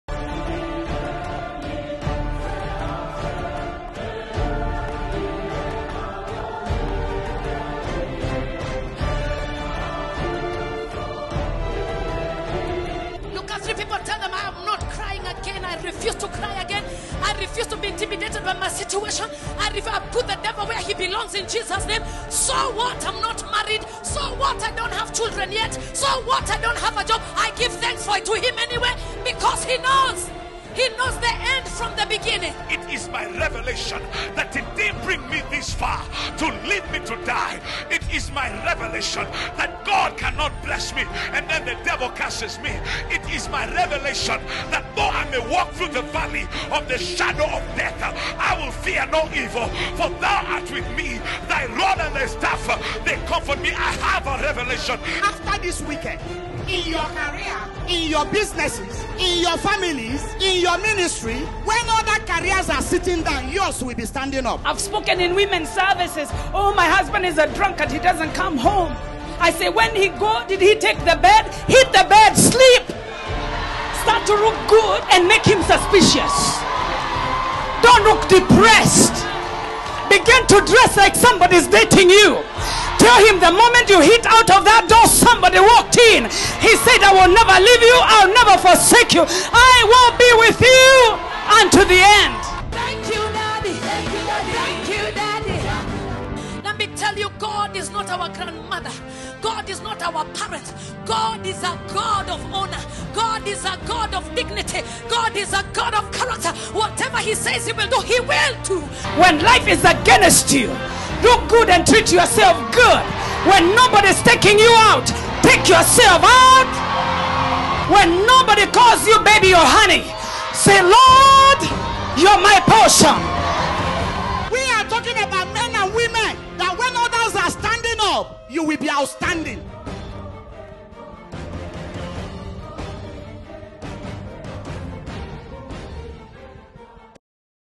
THE TRANSFORMED WOMAN CONVENTION .mp4
THE TRANSFORMED WOMAN CONVENTION will kick off this August 30th - Sept. 3rd 2011 @ Jubilee Christian Church Parklands Hosted by the one & only Rev. Kathy Kiuna.You cannot miss this ladies convention. Ministering will be Bishop Allan Kiuna, Rev. Teresa Wairimu & Pastor Esther Obasike among others. The evening sessions will be open to all from 4pm. Save the dates ladies